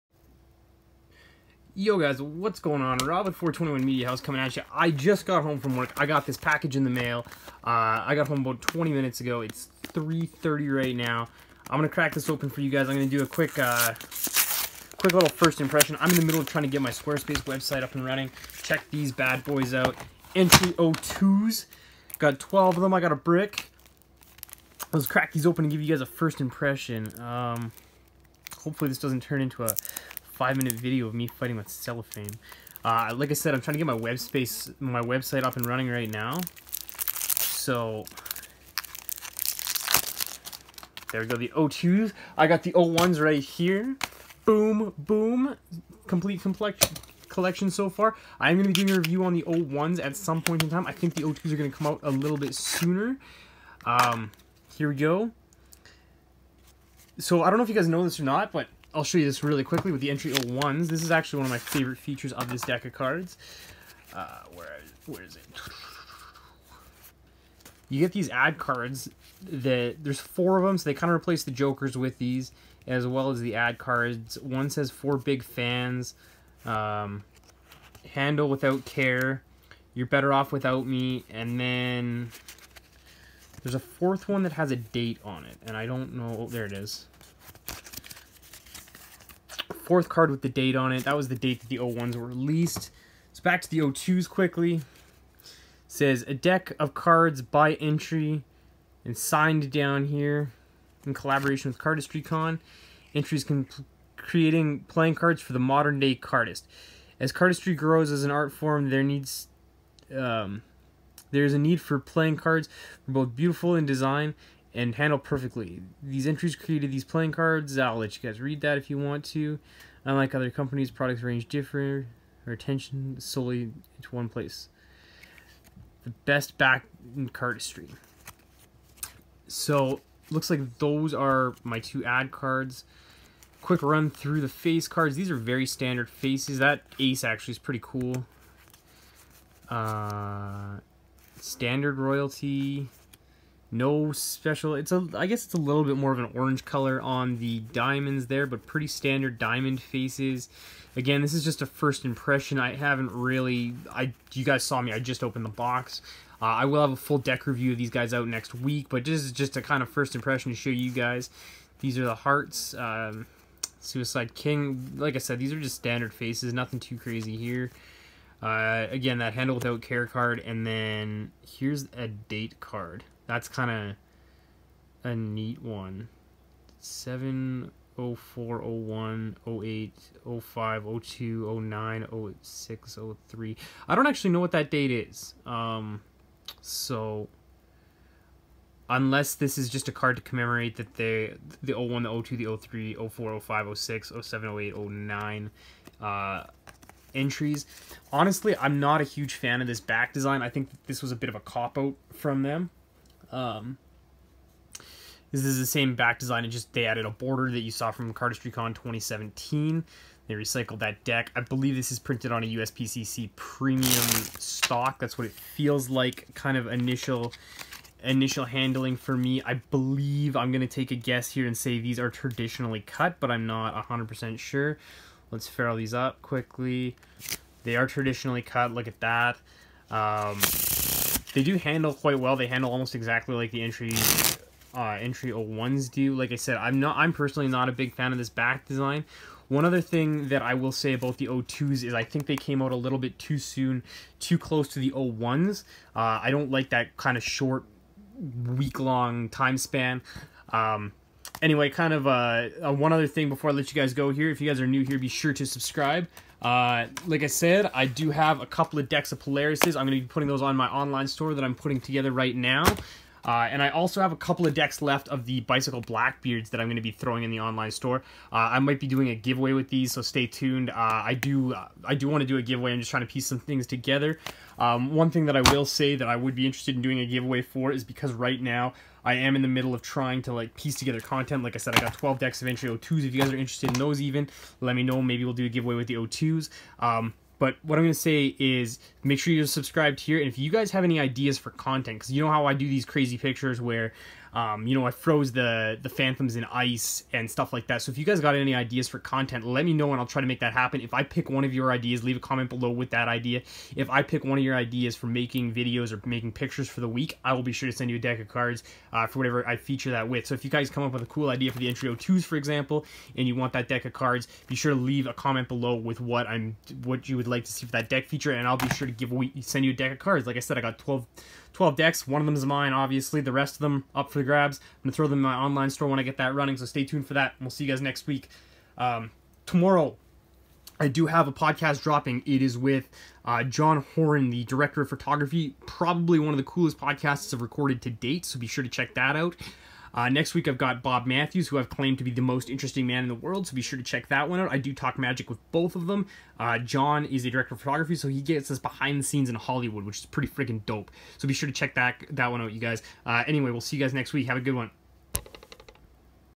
First Look - Entry 02 and Giveaway!
I just got a box of Entry 02's in the mail! watch as I take a first look and give you guys my first impression, I'll have a full review next week.

as far as the give away goes, I'm looking for ideas for the deck feature on Instagram next week.
rules
2. If i choose your idea I can choose between 0 and 3 submissions for a picture I will contact you and send you a deck of Entry 02's in the mail at no cost to you.
3. you must submit an original idea for the weekly deck feature I do on Instagram.
example of an non original idea would be do a werm cut.
example of an original idea (i already did this) freeze a card in a block of ice)
5. Contest closes Aug 24th 2018.